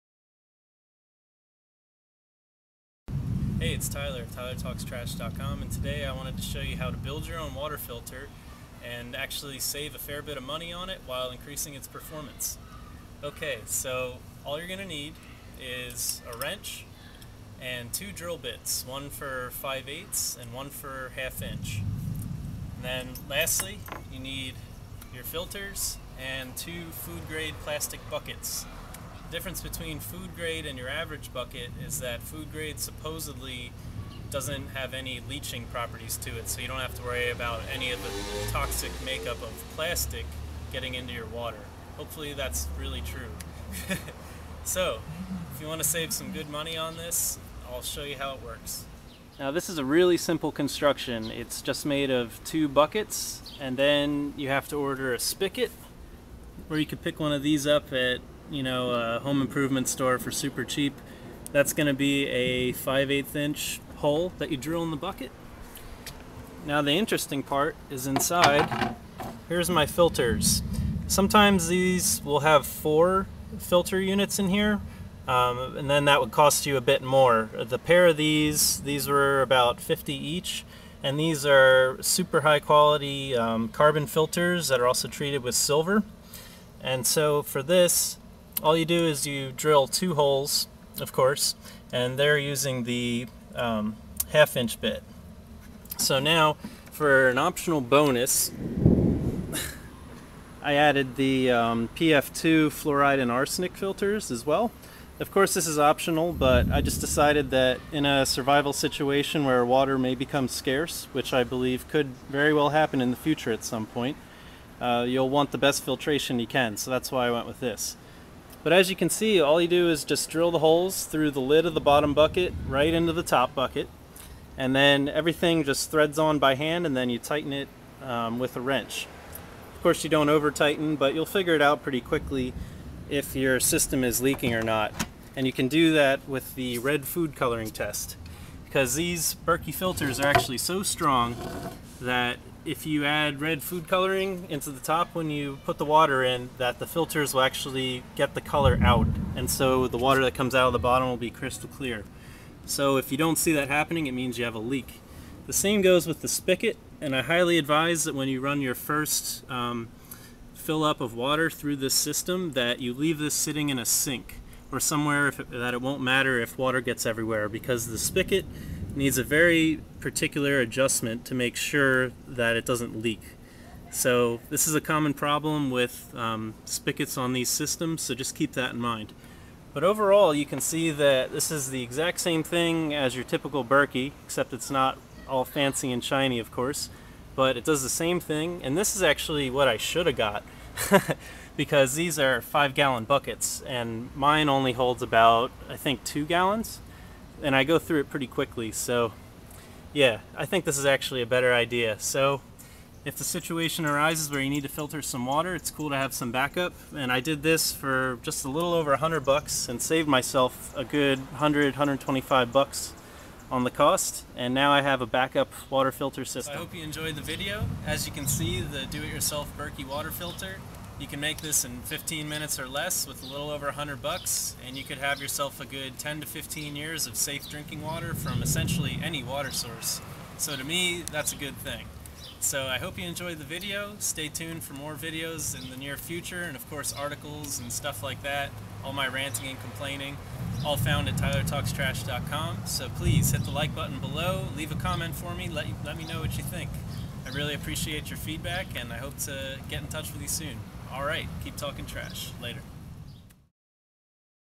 How to Build Your Own Water Filter
Lately I've been catching myself reading a lot of survival stuff, more specifically about water filtration.  I think that at some point in the near future, the grid will shut down for a substantial period of time, and water will become even more important than it already is.

 As a result, I decided to build my own water filter.  I used just two 5 gallon food grade plastic buckets and 2 Black Berkey purification elements paired with 2 Berkey Arsenic/Fluoride elements.  With this setup, you save money on the costly (but pretty) Berkey upper and lower chambers while expanding your water collection capacity.

Before I go any further, I wanted to make clear that I am NOT connected with Berkey in any way.  I just use their stuff and I like it.  All righty, where were we?

First, the buckets.  Go to your local grocery store and ask them if they have any 5 gallon food grade buckets...chances are that they'll be happy to give them to you.  Wash out the buckets.

Next, drill a 5/8" hole in the base of the bottom bucket for the spigot.  Thread on the spigot by hand, then tighten down using a wrench.  This is a tricky adjustment, so I recommend when you fill up your system for the first time, you place the system in your sink in case it leaks.  If it does leak, tighten it a little bit more.

Next, follow Berkey's instructions and prime your filter elements.  Essentially you're running water through them in reverse in order to clear out the process dust and open up the micropores of the filters.  This takes less than two minutes per filter.

Finally, drill two 1/2" holes through the base of the top bucket into the bottom bucket's lid.  Then, install the Black Berkey elements by simply threading them on nice and tight by hand.  The optional Arsenic/Fluoride elements are simply threaded onto the base of the Black Berkey elements.

Run a full cycle of filtered water through the system and dump it...it'll taste a bit metallic.  From here on out, you have up to 6,000 gallons with the Black Berkey elements, and 1,000 gallons with the optional Arsenic/Fluoride elements.

This is an easy project that will ensure the safety of you and yours for a long time when disaster strikes.